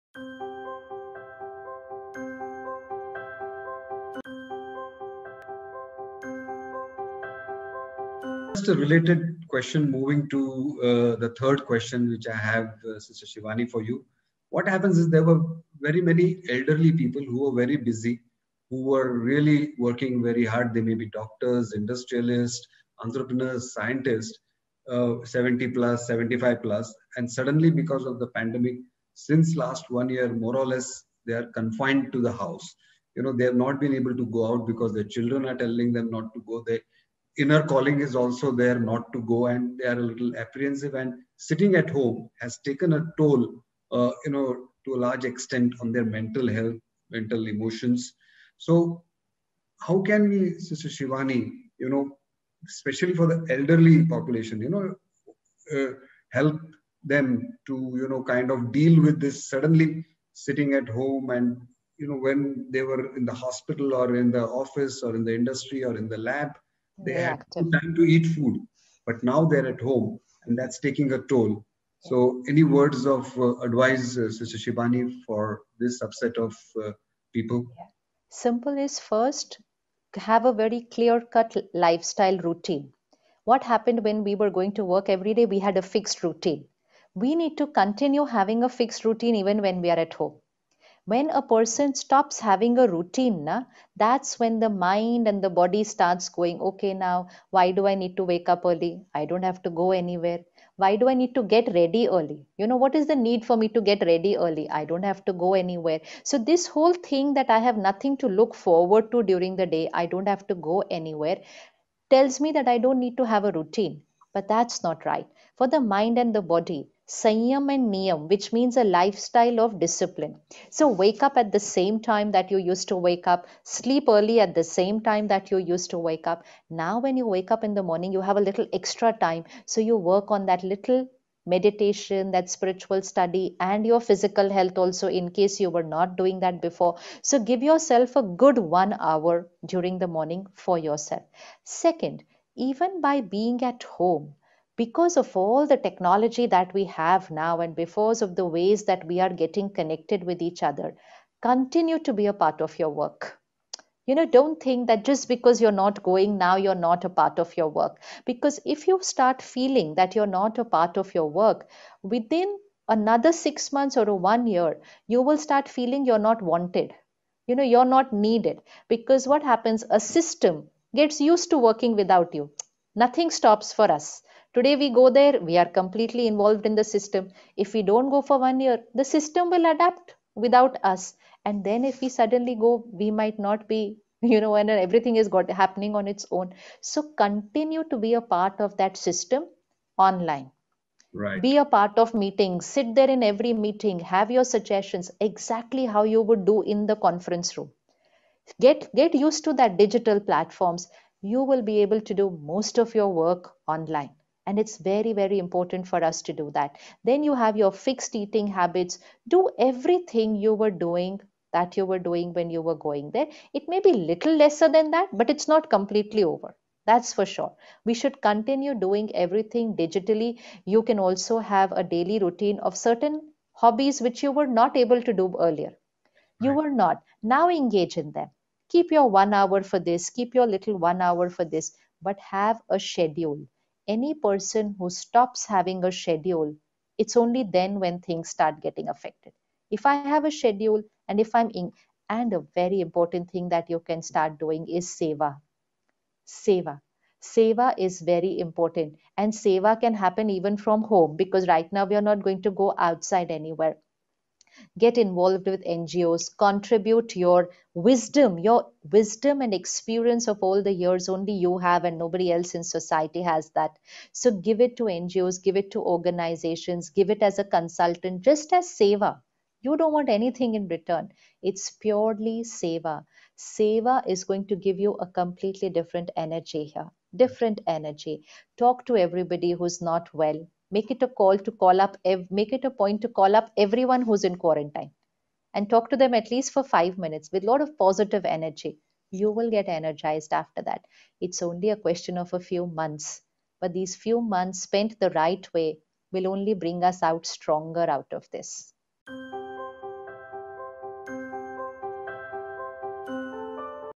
How to deal with change in daily lifestyle Answered by BK Shivani in Conversation with Dr. Parag
How to deal with change in daily lifestyle
Answered by BK Shivani in Conversation with Dr. Parag